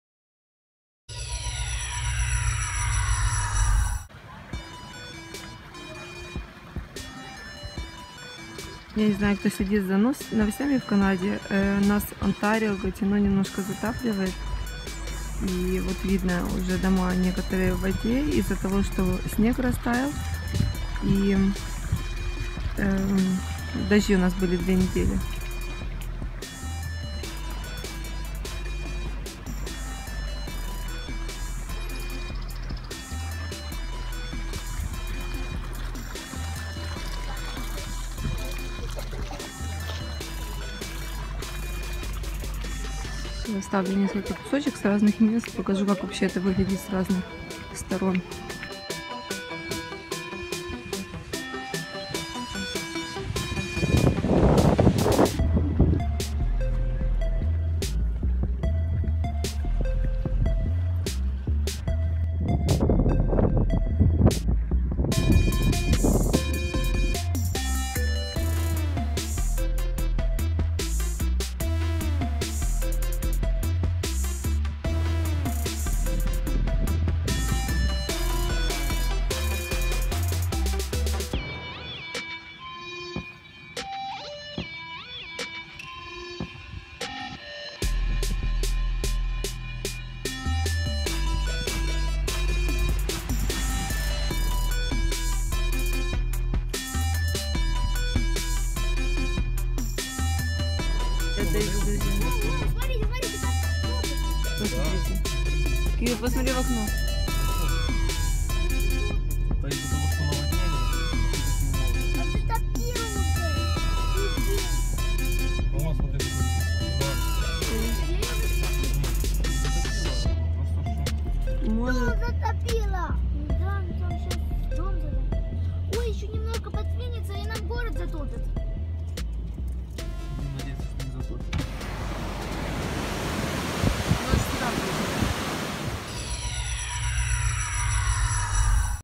Нас затапливает! | Канада #154
Всем привет! У нас стихийное бедствие! Много людей уже эвакуировали, от нас вода в 2 км... Уже пришла армия на помощь и много людей волонтерами помогают спасать подтопленные дома